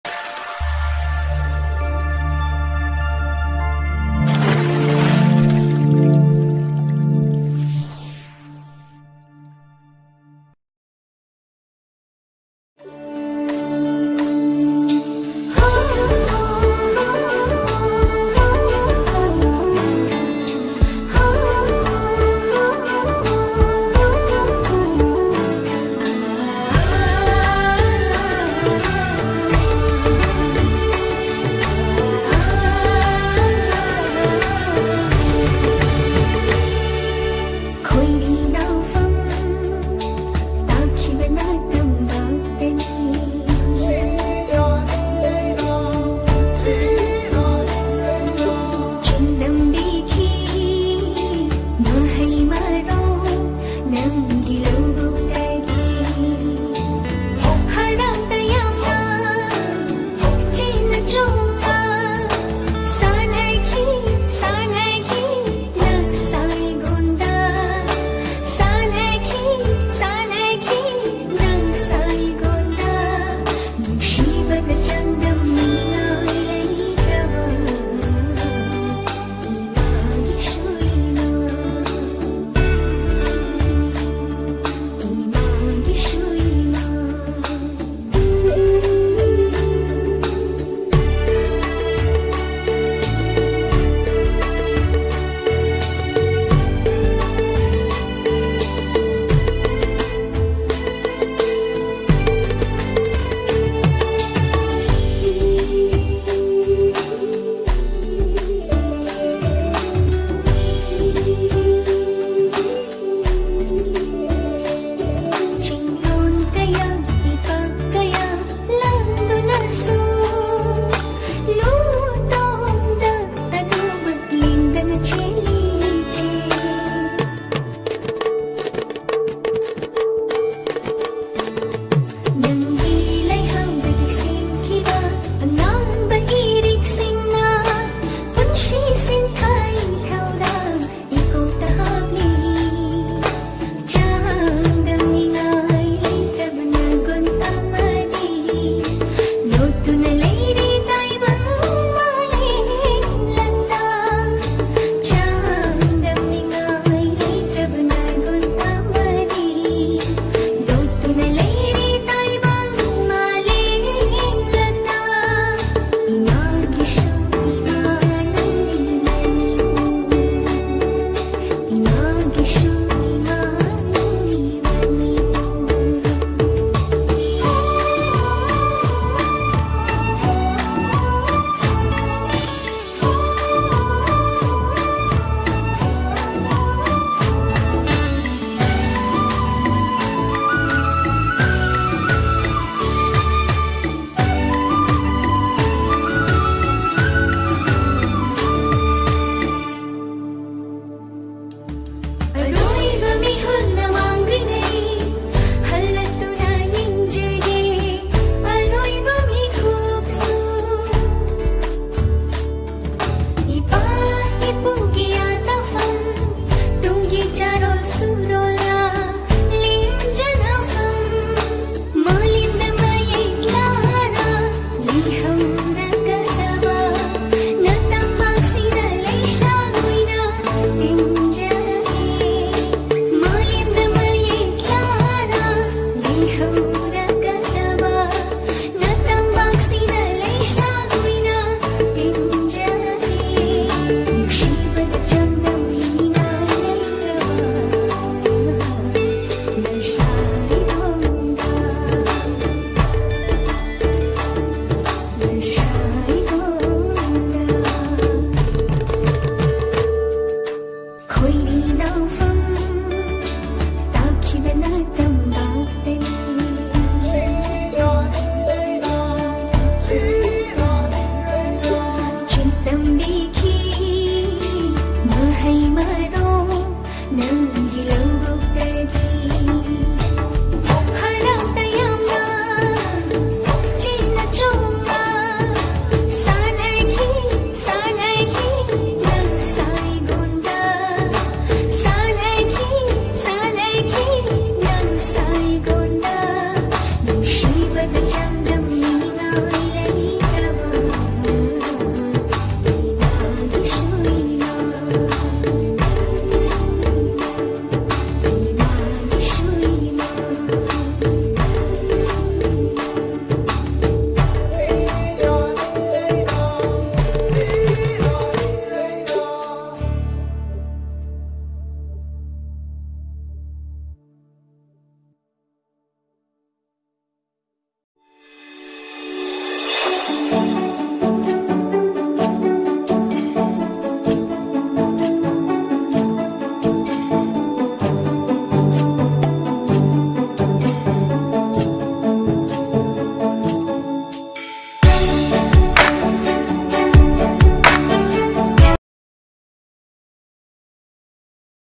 Soyam Jeny Devi From Yairipok || 2nd Round nachom leirang online dance competition 2022
2nd State Level Nachom Leirang Online Dance Competition-2022
                             SI. NO:
C

Address:

Song:

Institutions:

Voting for best popular people's  choice award based on most view video.